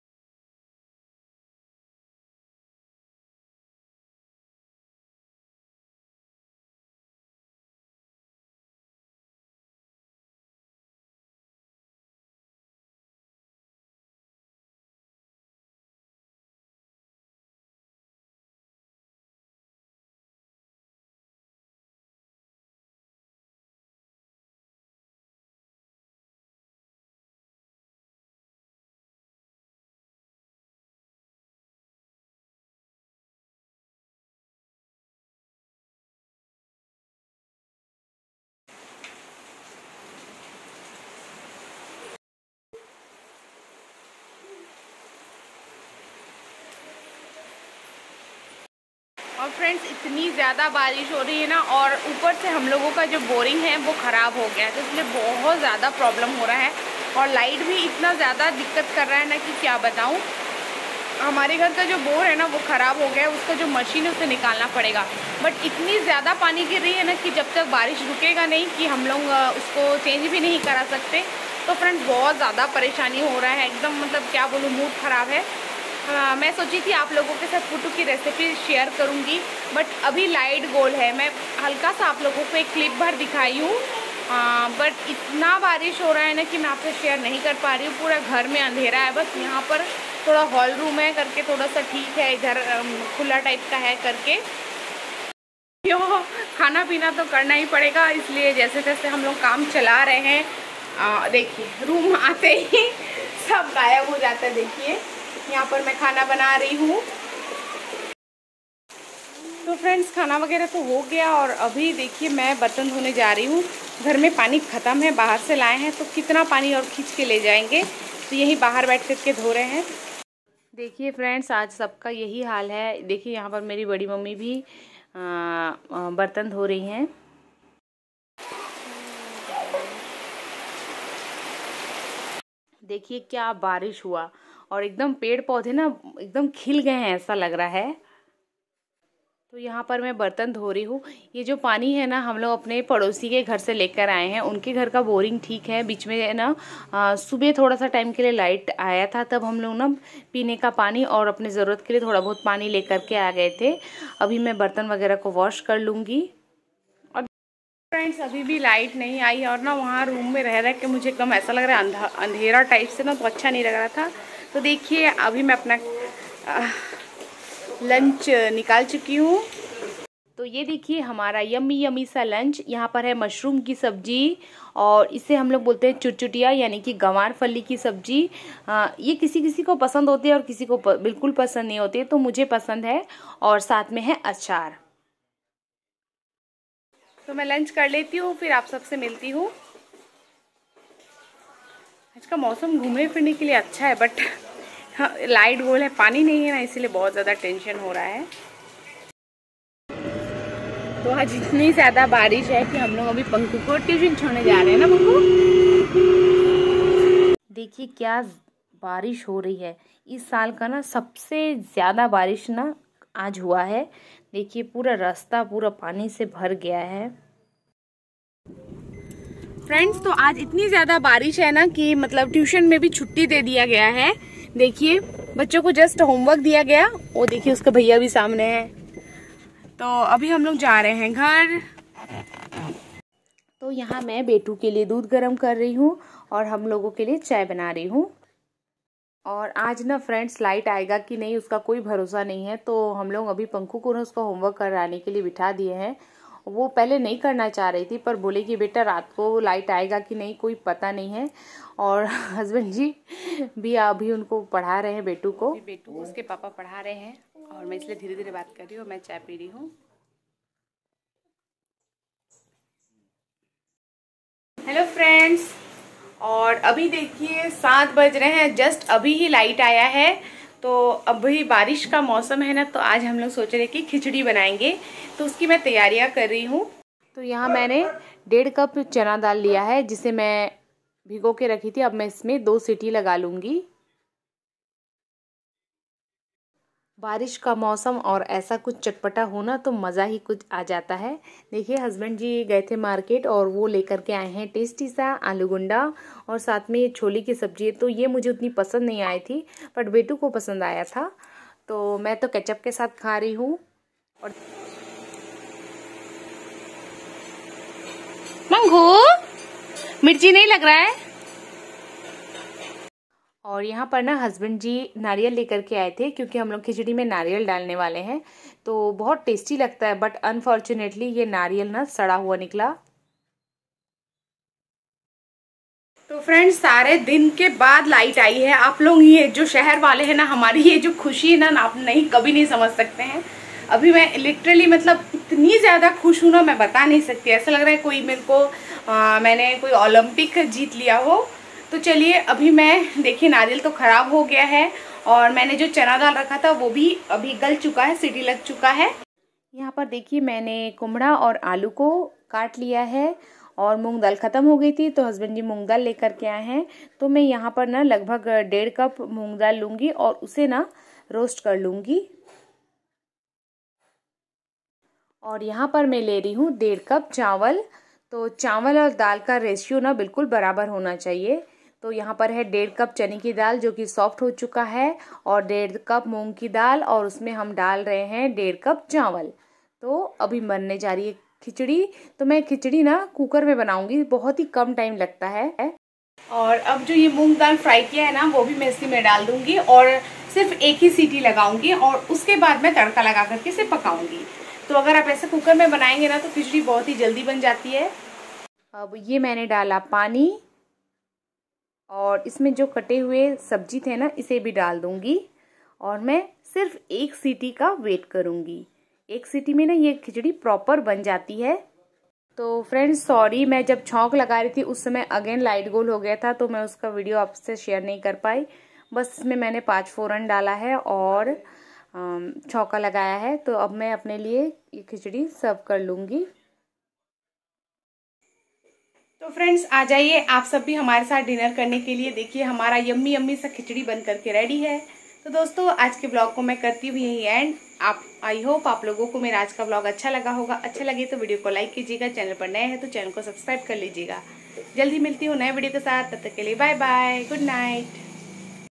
भारी बारिश, दिन भर लाइट नहीं है और हमारा बोरवेल भी खराब हो गया है। heavy rain ⛈️के कारण school बंद
hello friends